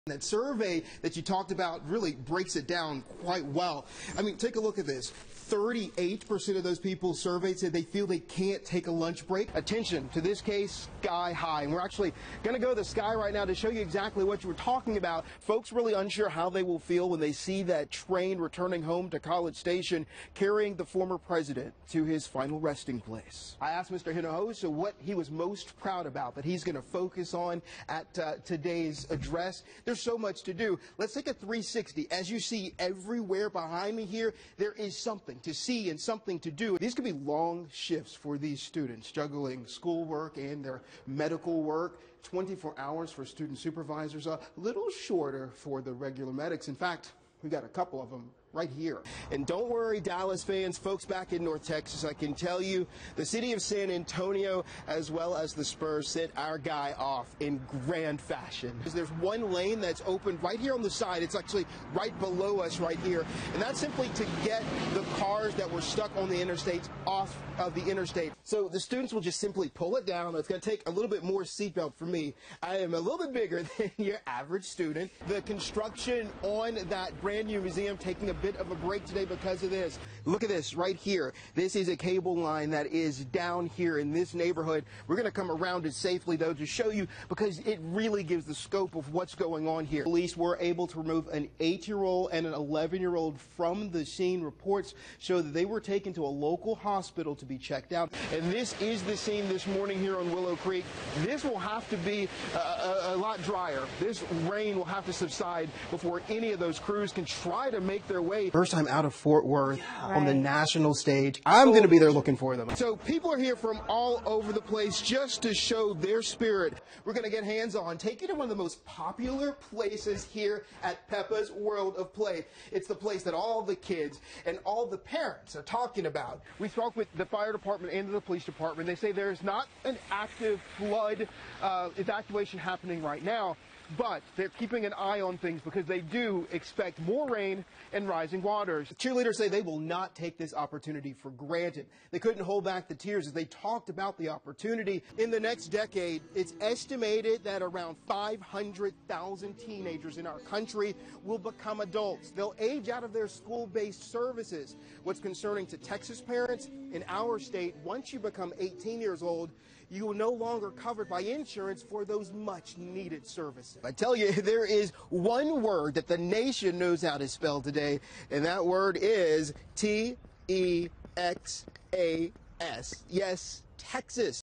Larry Collins reporter highlights: Quick Look
Larry Collins has been a reporter for KXAS in Dallas, Texas, since 2016. He has previous experience working as a reporter at WBFF in Baltimore, Maryland.